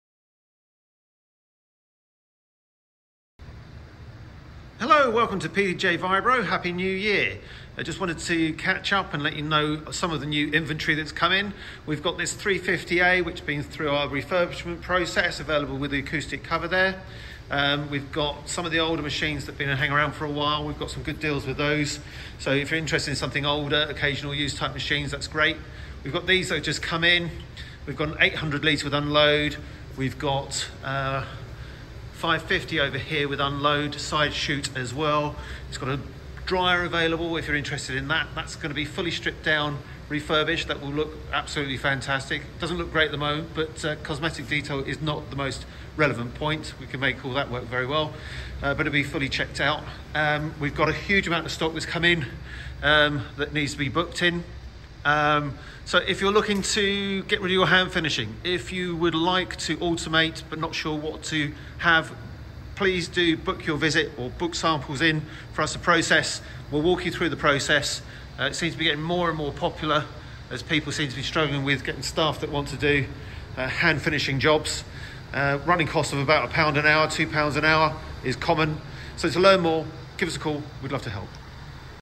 New Year Inventory Catch-Up
Happy New Year! This week, we have an inventory catch-up. We have a 350-A vibratory bowl finishing machine that has been through our refurbishment process. There are some great deals on some of the older, occasional-use type machines. Machines that have just come in, which will be fully stripped down and refurbished, include an 800-litre machine with unload, a 550-litre machine with unload, a side chute, and a dryer available for it. We have a huge amount of new stock that has arrived, so if you're looking to get rid of your hand finishing and automate your finishing process, book your samples in and we'll walk you through the process. Automated hand finishing is becoming increasingly popular as companies are struggling to find staff for hand finishing jobs, running costs of £1-£2 per hour is common.